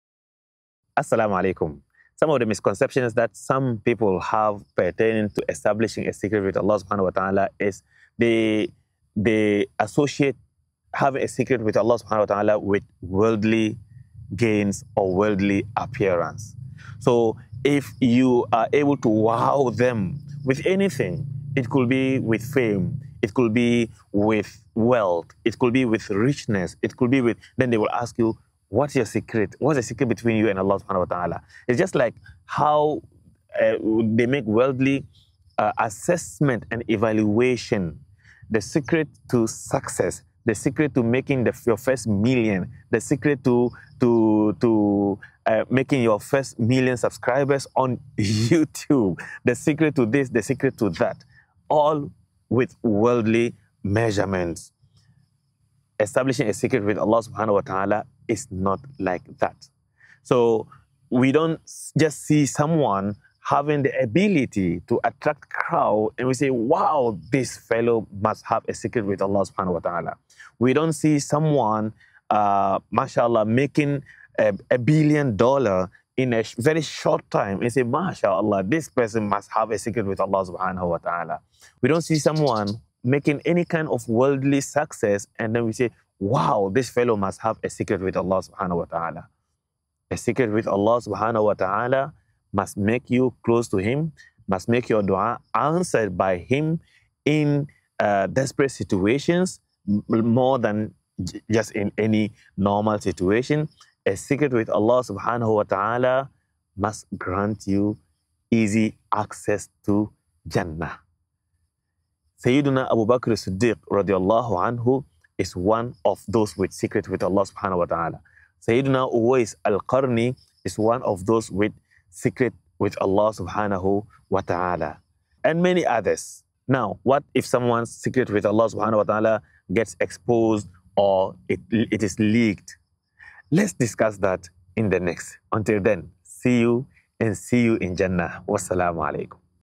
A Secret with Allah - Part 3: Misconceptions Regarding Establishing a Secret with Allah
In these parts of the short clips, we are learning to establish a secret with Allah, the Islamic way. This is Part 3.

🌟 Al-Bushra by GSalam 👉🏾👉🏾   ￼ / @youralbushra
🌟  TVQ.Academy 👉🏾👉🏾   ￼ / tvqacademy